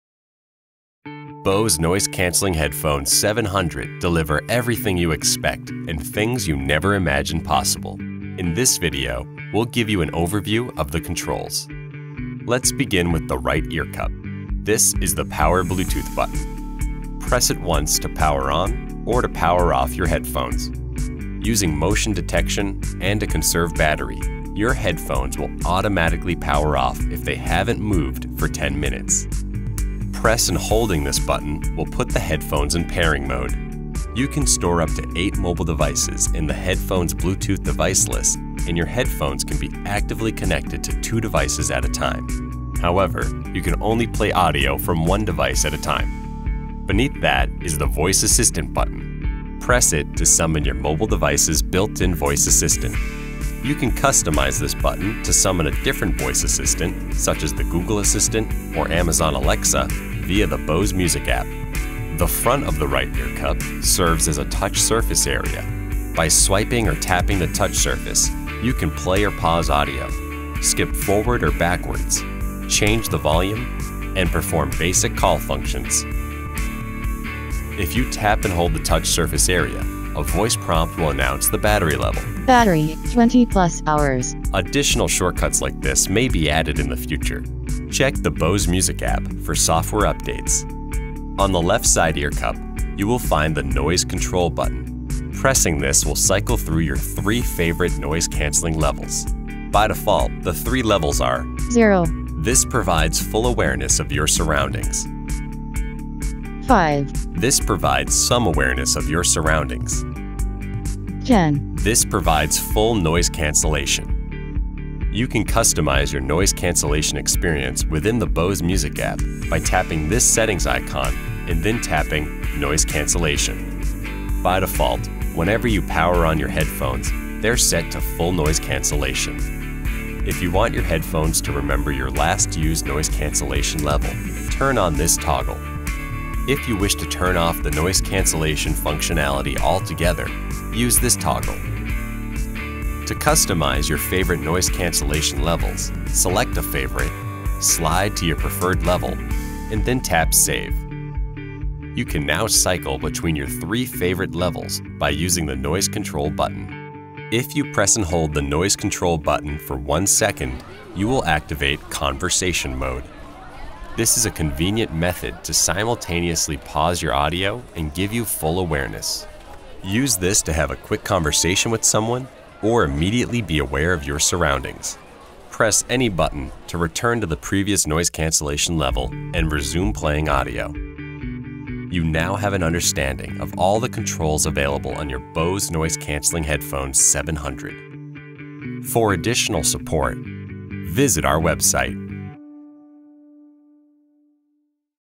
Bose Noise Cancelling Headphones 700 – Controls Overview
In this video, we'll give you an overview of the controls and features of the Bose Noise Cancelling Headphones 700.

0:00 Intro
0:12 Power/Bluetooth Button
0:46 Voice Assistant Button
1:01 Touch Surface Area
1:30 Noise Control Button
1:54 Customize Noise Cancellation Settings
2:36 Conversation Mode
3:01 Outro